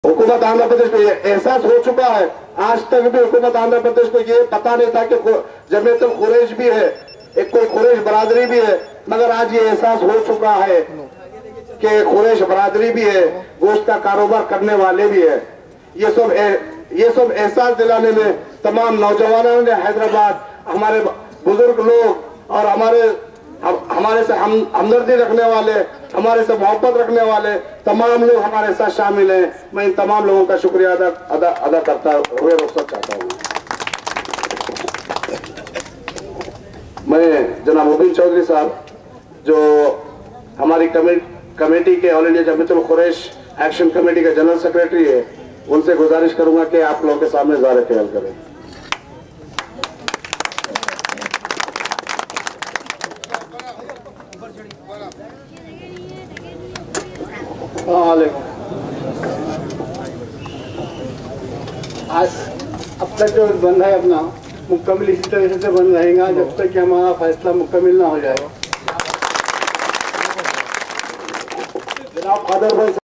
HUNGER STRIKE BY ALL INDIA JAMIATUL QURESH ACTION COMMITTEE UNDER LEADERSHIP OF FAHEEM QURESHI ADV
HUNGER STRIKE ORGANIZED BY ALL INDIA JAMIATUL QURESH ACTION COMMITTEE UNDER THE LEADERSHIP OF ITS PRESIDENT FAHEEM QURESHI ADVOCATE STRONGLY PROTESTING AGAINST THE DECISION OF CHANDRABABU NAIDU THE THEN CHIEF MINISTER OF ANDHRA PRADESH===THE THEN GENERAL SECRETARY MUBEEN CHOUDHRAY, VIDE PRESIDENTS QADEER QURESHI AND ABDUL QADER QURESHI, MOHD HUSSAIN QURESHI AND OTHERS CONTROLLED THE AGITATION - HARI LAL PAHELWAN AND OTHER GUESTS CAN BE SEEN SEEN ATTENDING THE PROTEST